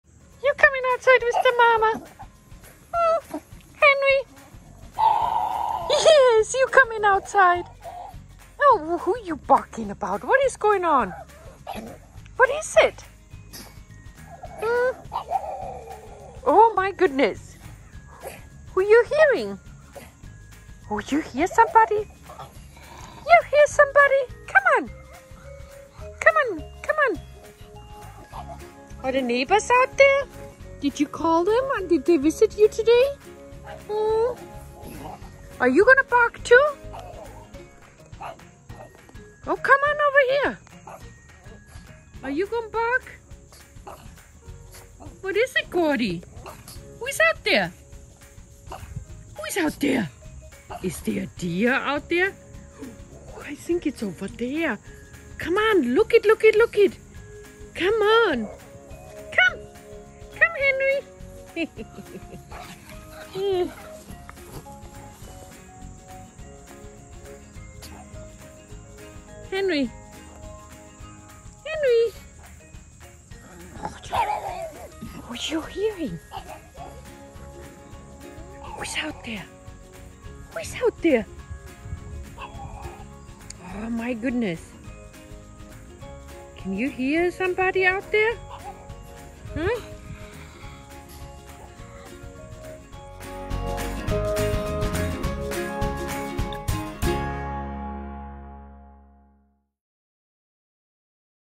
When a Pug hears something
Mom has been working more than usual and Henry is overly excited he even get’s Gordon to join barking.